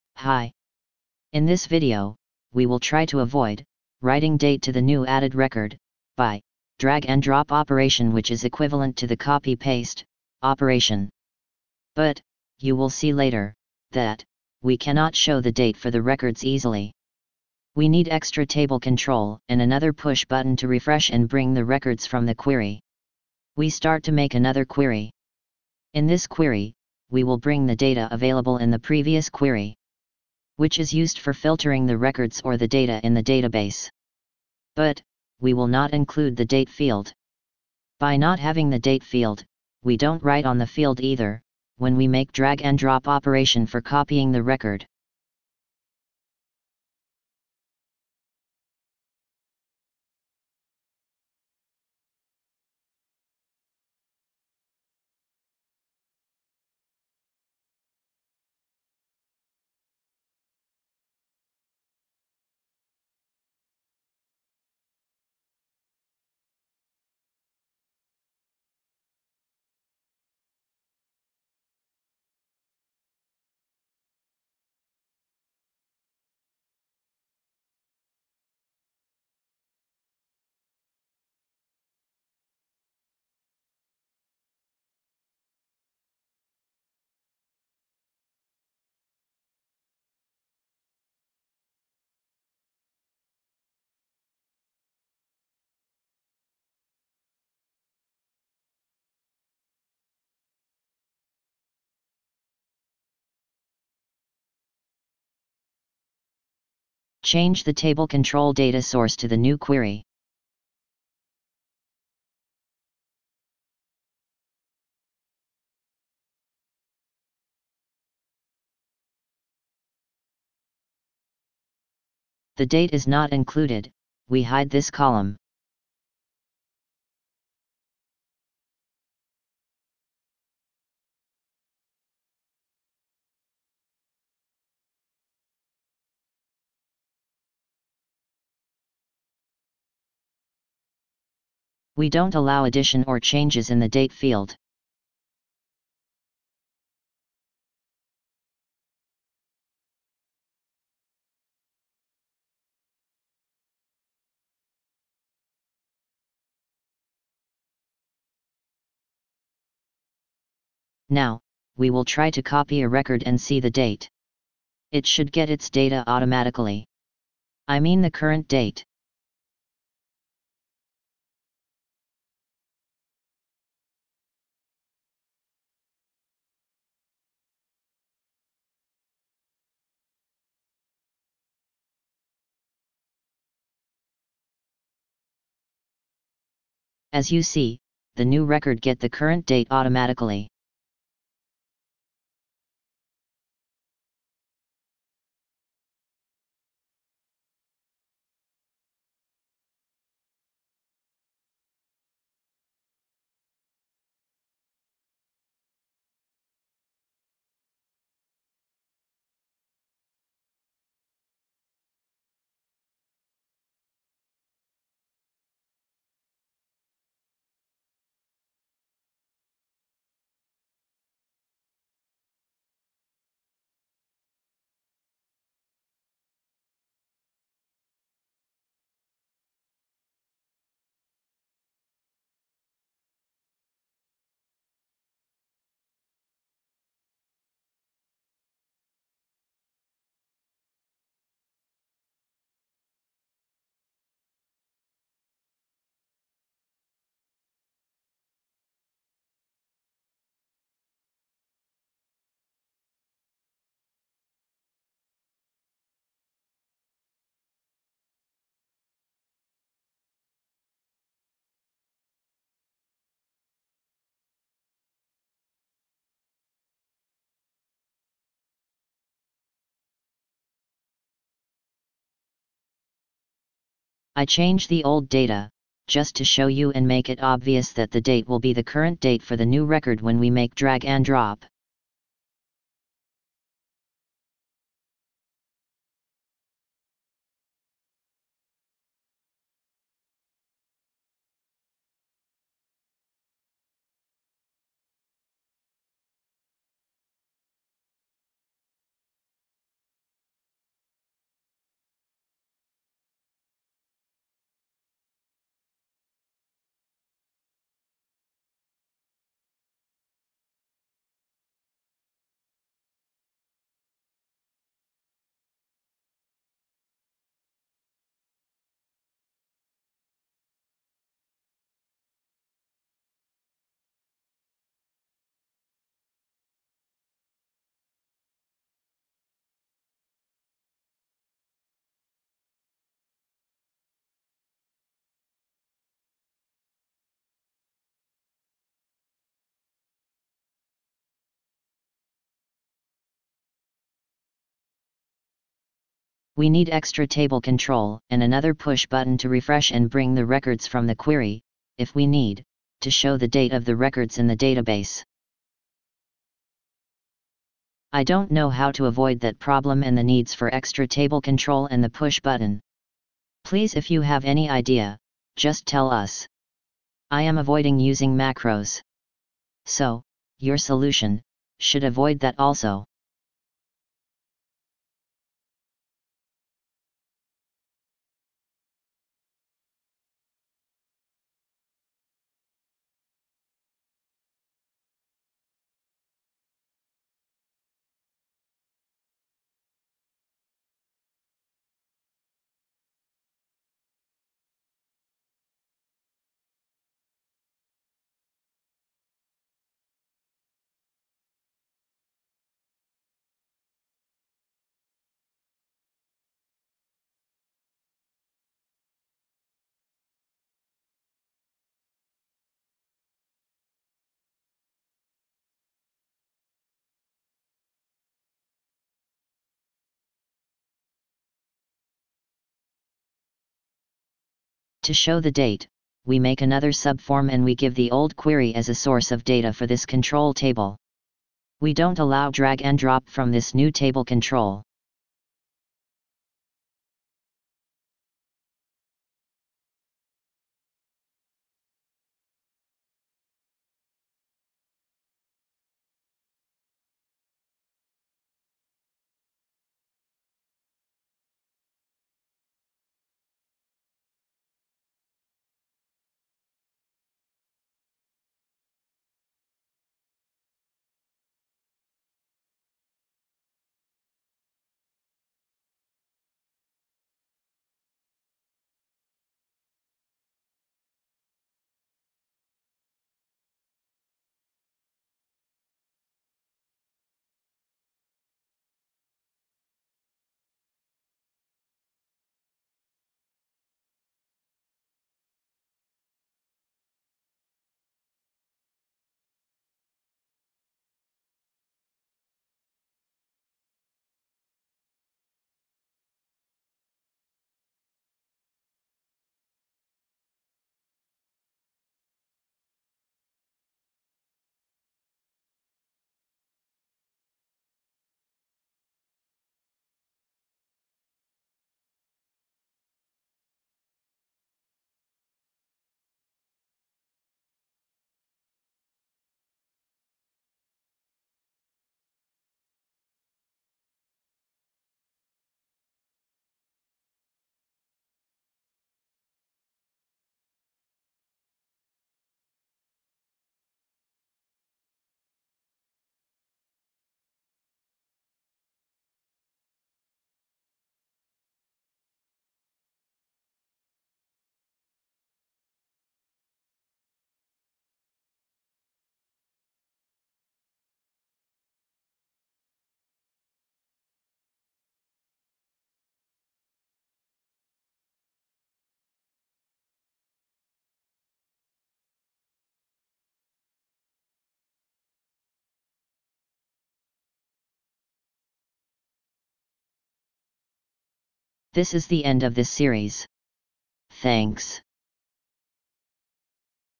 LibroOffice Base - Part 3 (Avoid copying a column when drag-and-drop made)
In this part, we will avoid Writing to the date column.. Since the date was auto, we shouldn't write to it. But, we will not be abile to show the date for the records easily.